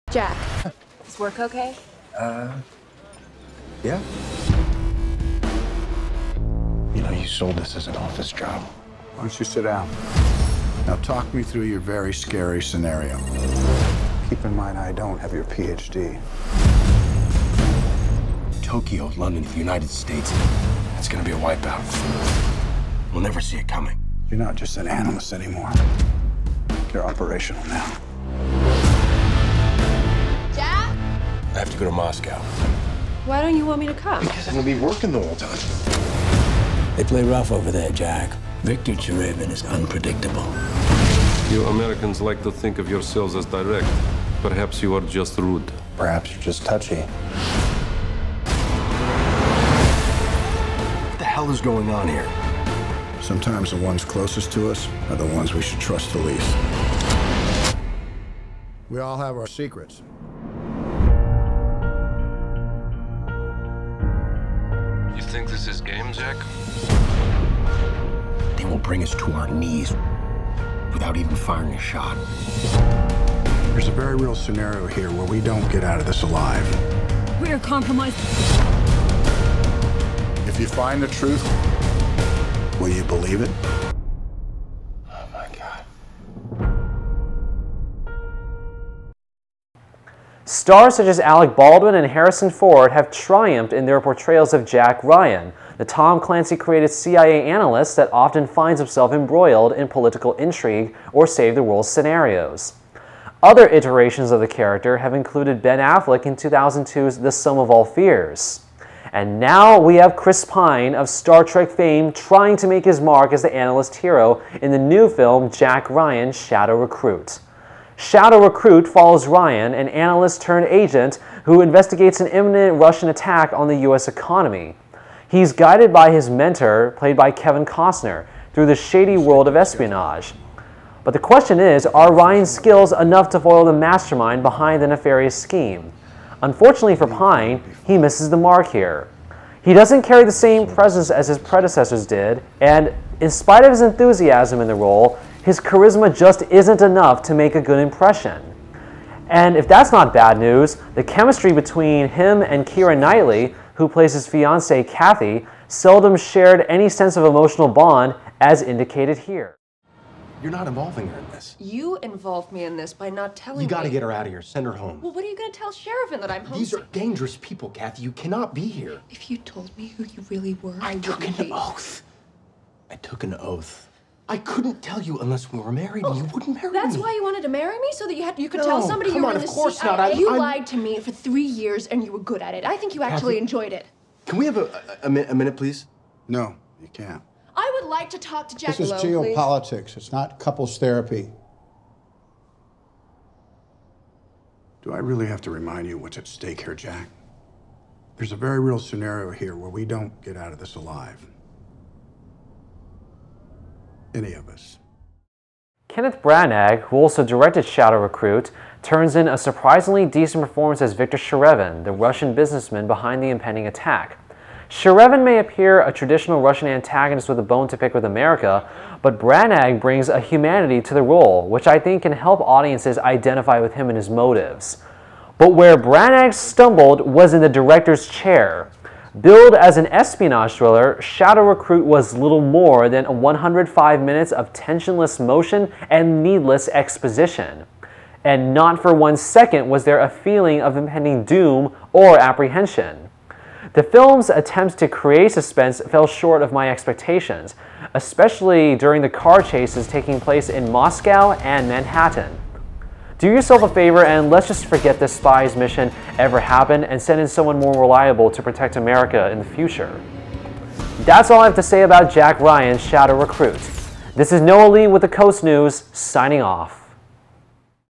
Jack Ryan: Shadow Recruit
Film critic Noah Lee reviews the latest film based on Tom Clancy's character Jack Ryan.